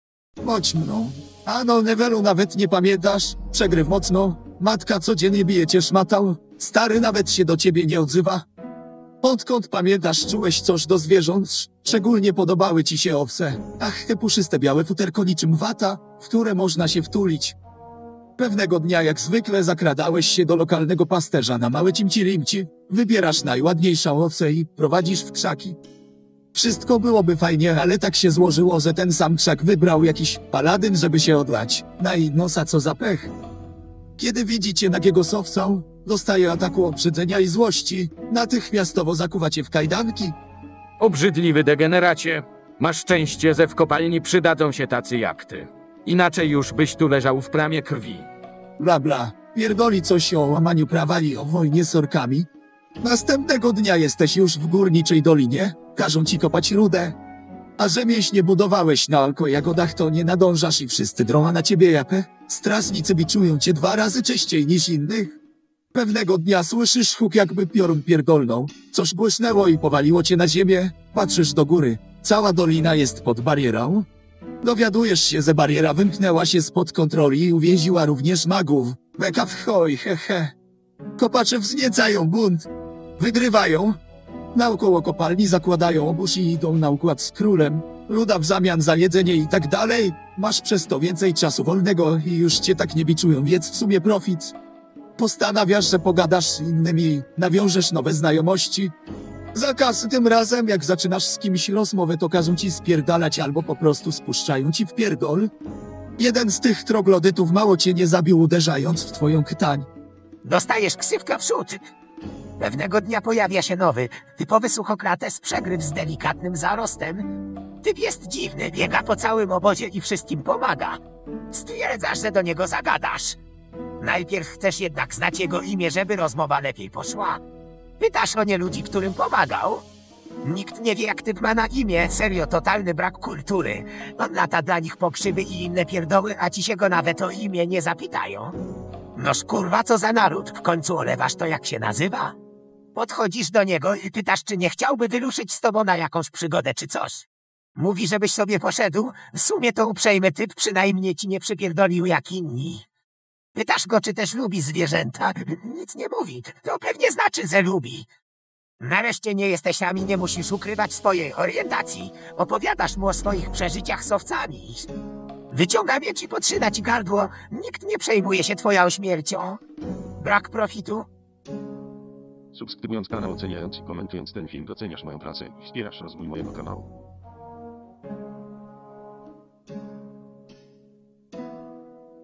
Gothic pasta - Przegryw Wrzód (czyta SI)
Wrzód, znowu ten Wrzód... :D
Oto kolejna pasta inspirowana grą

Głos czytający pastę, który słyszysz na filmie ZOSTAŁ WYGENEROWANY PRZEZ SZTUCZNĄ INTELIGENCJĘ za pomocą ogólnodostępnej darmowej technologii tacotron2 i waveglow należącej do Nvidia.

Za próbki dźwiękowe potrzebne do nauki syntezy mowy posłużyły dialogi z gry wypowiadane przez danego aktora głosowego.
Jeśli aktor głosowy dialogów z gry użytych do nauki sztucznego głosu ma jakieś obiekcje odnośnie użycia takiego wytrenowanego sztucznego głosu (na podstawie Jego własnego prawdziwego głosu) do przeczytania treści zawartej w niniejszym filmie, bądź czuje się tym urażony albo uważa, że narusza to Jego dobre imię i nie godzi się na to, proszę o kontakt w tej sprawie, a film zostanie natychmiastowo usunięty.